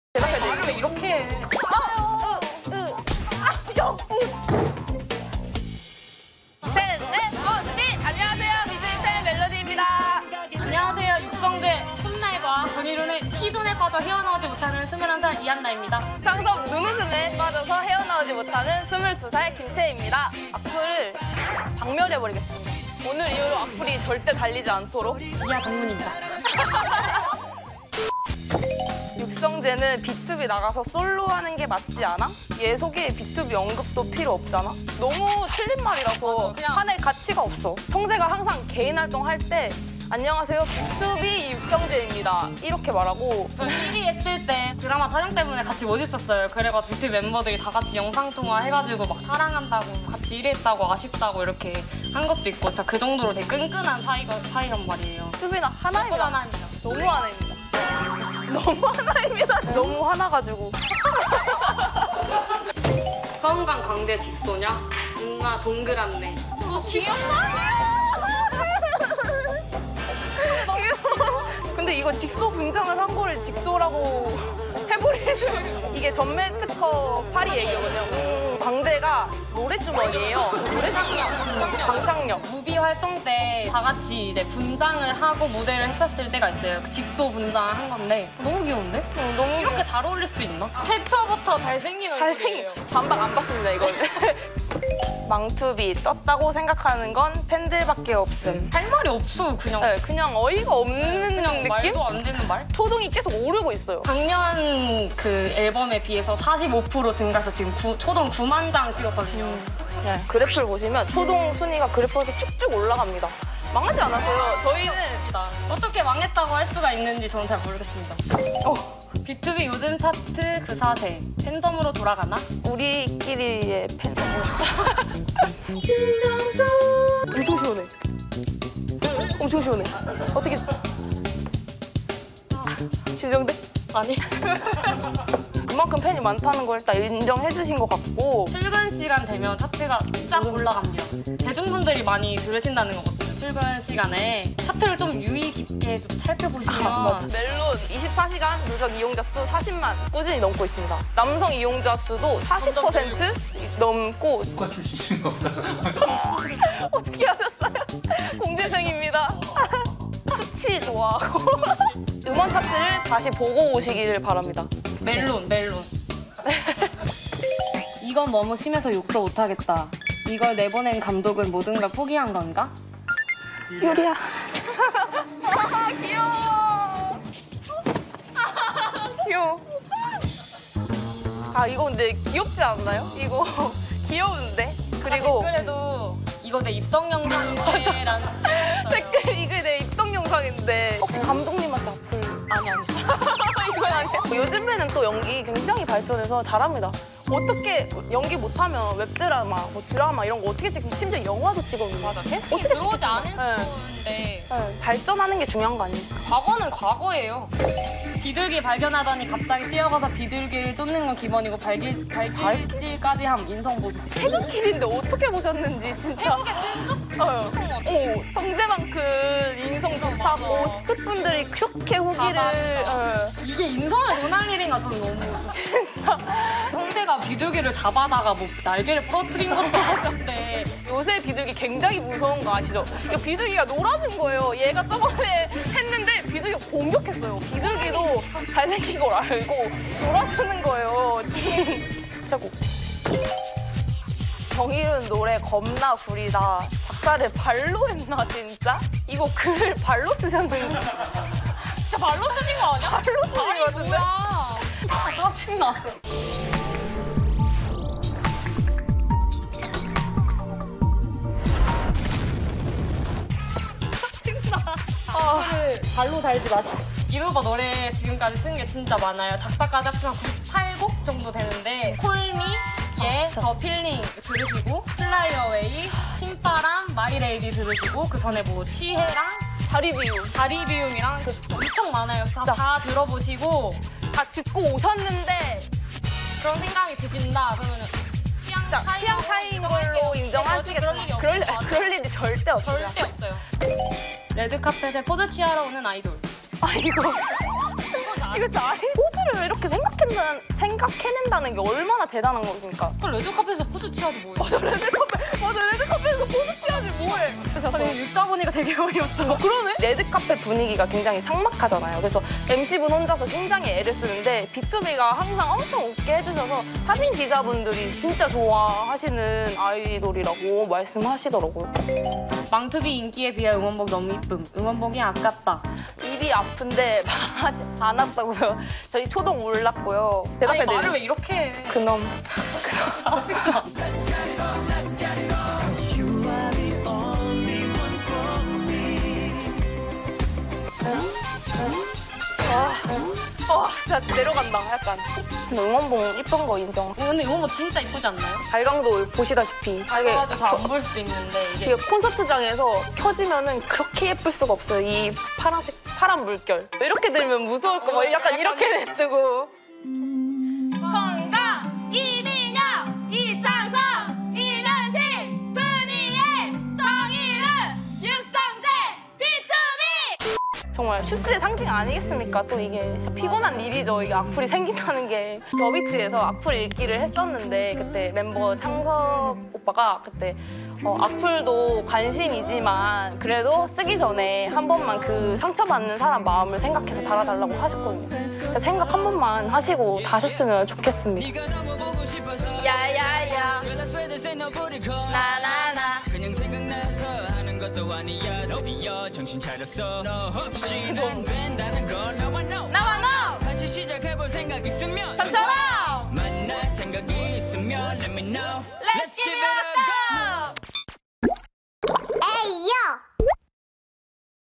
[Vietsub] Melody đập tan các comment ác ý - Cùng BTOB tiếp tục hành trình năm thứ 7
Cre: AYO 에이요
Vietsub: Bìm Bịp Bông

BÌM BỊP BÔNG là kênh YouTube của một BTOB fanblog cùng tên, hoạt động chủ yếu trên facebook.

BÌM BỊP BÔNG is a BTOB fanblog's YouTube channel